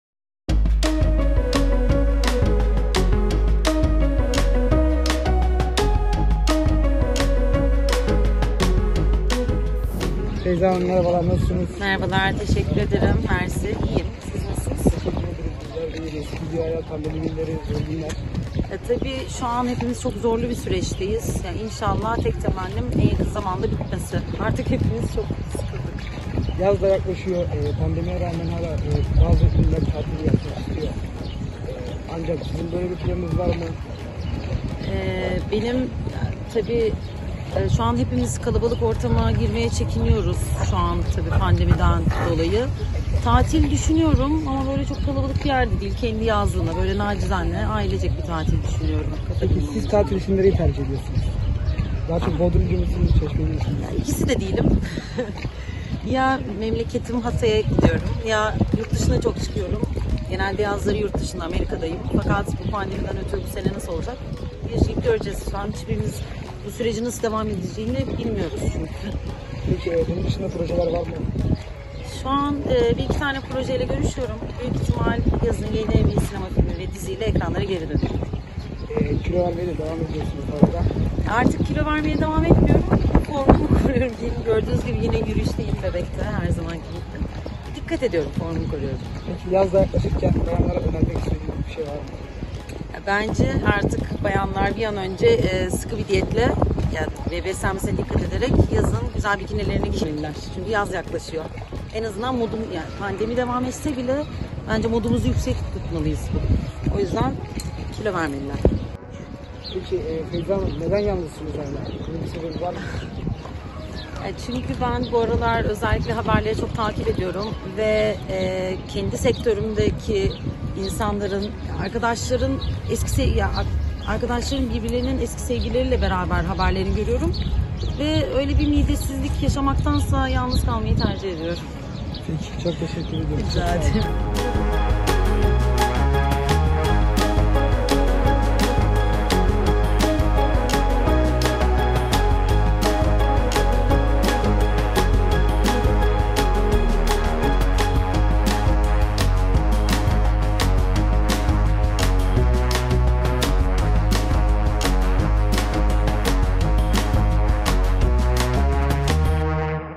Feyza Civelek'ten Bomba Aşk Açıklaması: Midesiz Buluyorum | YAZA YENİ PROJE İLE GİRECEK
Adını Feriha Koydum isimli dizide oynadığı Lara karakteriyle tanınan Feyza Civelek, Bebek'te yürüyüş yaparken magazinburada.net kameralarına yakalandı.

Feyza Civelek, formunu korumak için çıktığı Bebek yürüyüşünde magazinburada.net kameralarına yakalandı. Pandeminin zor geçtiğini söyleyen Civelek, 'Artık evde sıkıldık, yaz gelsin istiyorum' açıklaması yaptı.

Yaza formda girmek için spor yapmayı ihmal etmeyen Feyza Civelek, "Yazın kalabalık yerlerden uzak duracağım, daha çok kendi halimde ailemle yazlığımda tatil yapmak istiyorum. Belki memleketim Hatay'a giderim" dedi.

Adını Feriha Koydum, Güllerin Savaşı ve Bir Umut Yeter isimli projelerde aldığı rollerle tanınan Feyza Civelek, görüşme aşamasında olduğu birkaç proje olduğunu önümüzdeki dönemde bir dizi ya da sinema filmiyle ekranlarda olacağının müjdesini verdi.

Fit görüntüsüyle dikkat çeken Feyza Civelek, hemcinslerine de birtakım uyarılarda bulundu. Civelek, "Ben artık kilo vermiyorum formumu koruyorum. Bunun için Bebek'te yürüyüş yapıyorum. Kadınlar pandemi devam etse bile modunu yüksek tutmalı ve kilo vermeli ki yazın güzel güzel bikinilerini giyebilsin" şeklinde konuştu.

Feyza Civelek, neden aşk hayatını durgun olduğu konusunda ise, "Haberleri takip ediyorum, kendi sektörümdeki insanların arkadaşların birbirlerinin eski sevgilileriyle beraber haberlerini görüyorum. Böyle bir midesizlik yaşamaktansa yalnız kalmayı tercih ediyorum" diyerek sektördeki insanlara ağır bir göndermede bulundu.